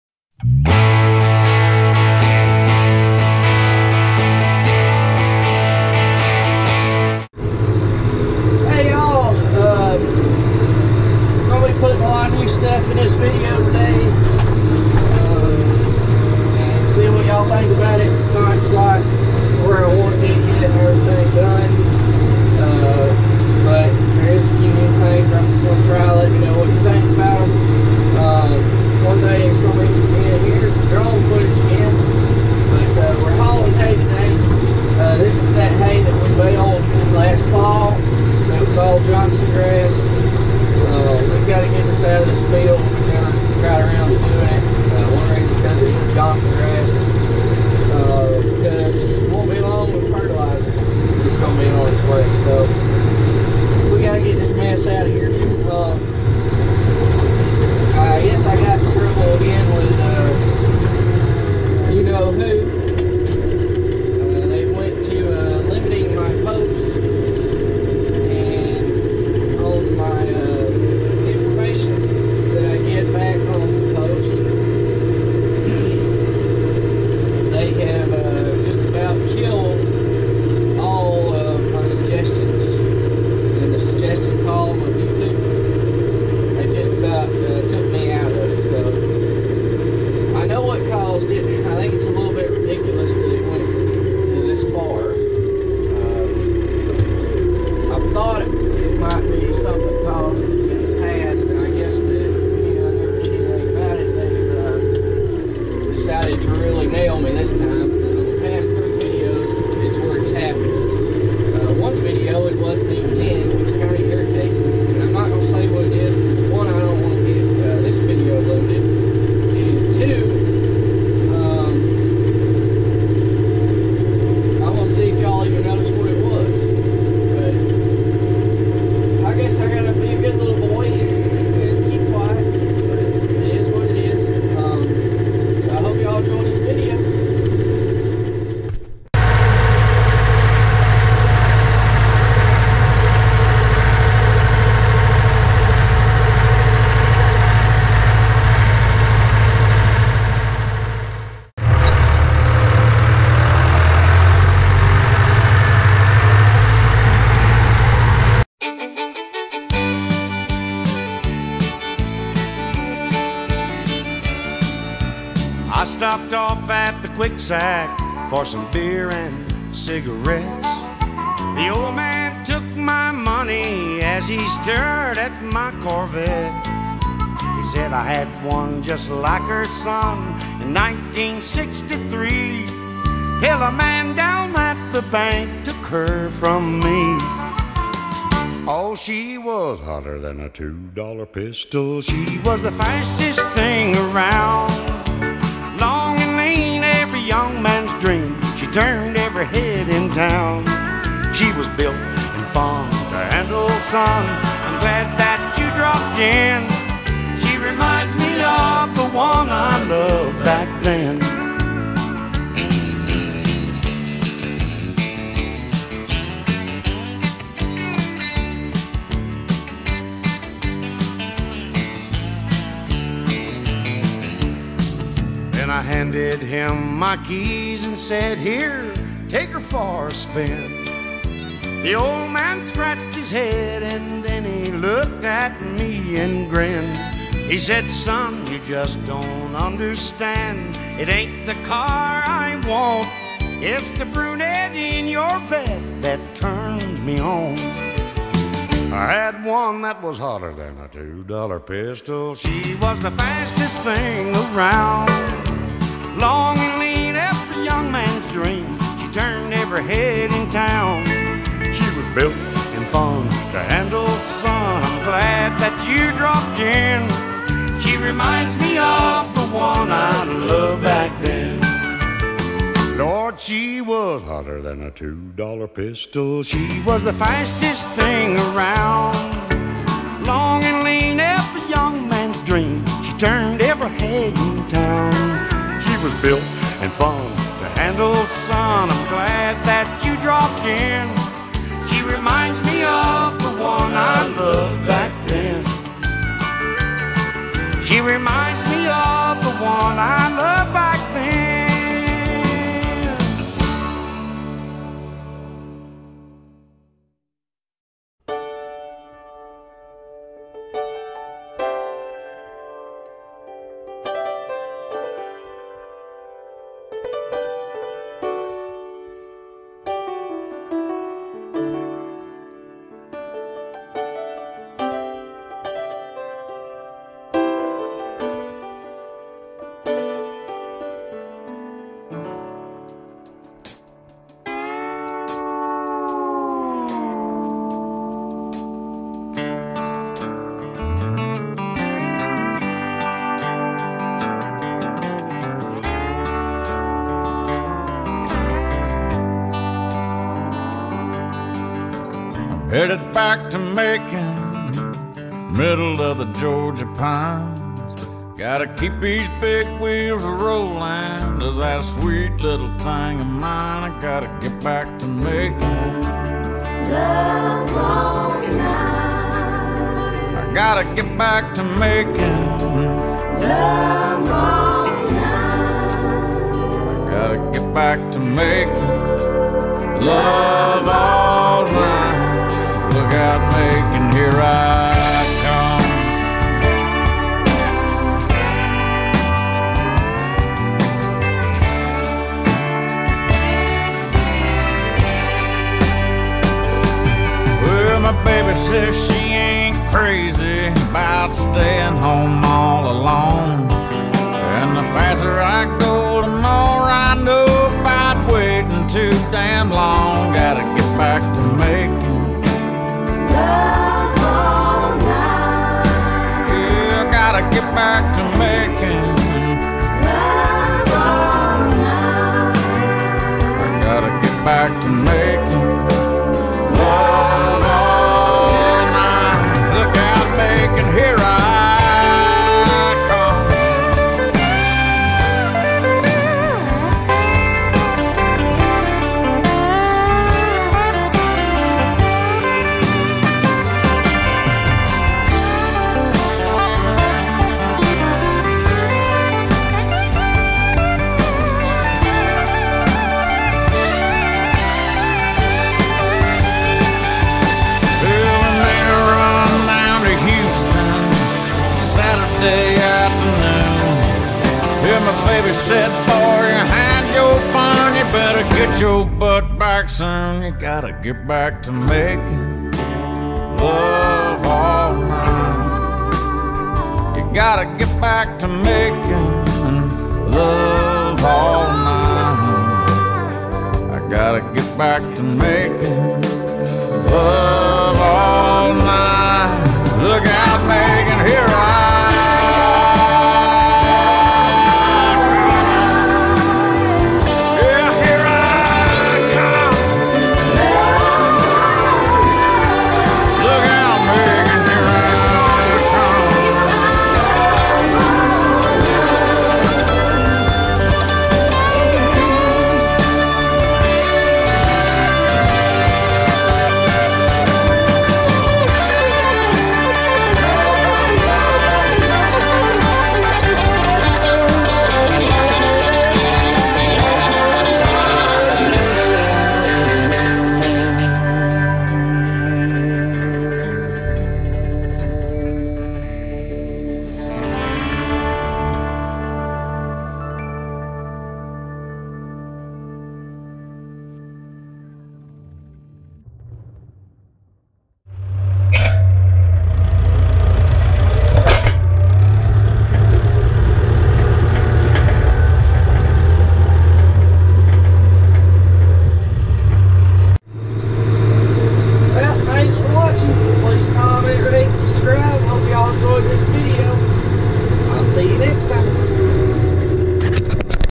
Loading a little hay with the 5400
Loading hay with the massey ferguson 5445 on the big tex an international with some drone footage